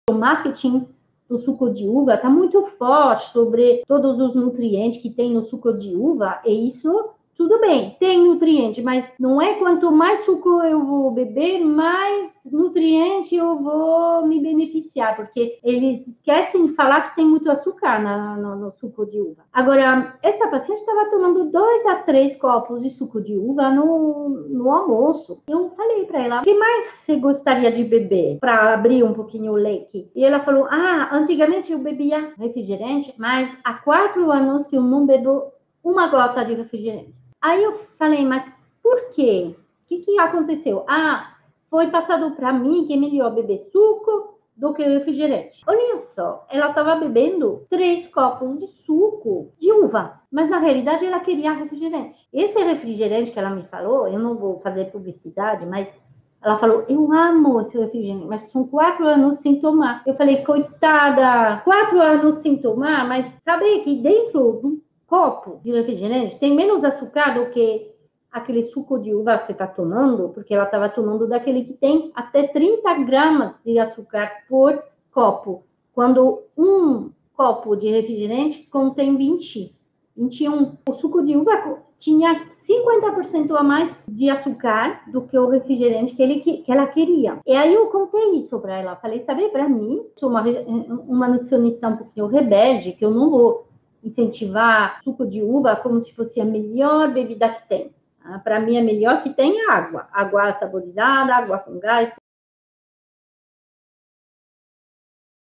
Suco não é sempre a melhor escolha
Suco não é sempre melhor do que refrigerante. Já vi uma paciente se forçando a tomar suco no lugar de refrigerante, só que bebia muito mais suco porque estava insatisfeita. Resultado: consumia muito mais açúcares.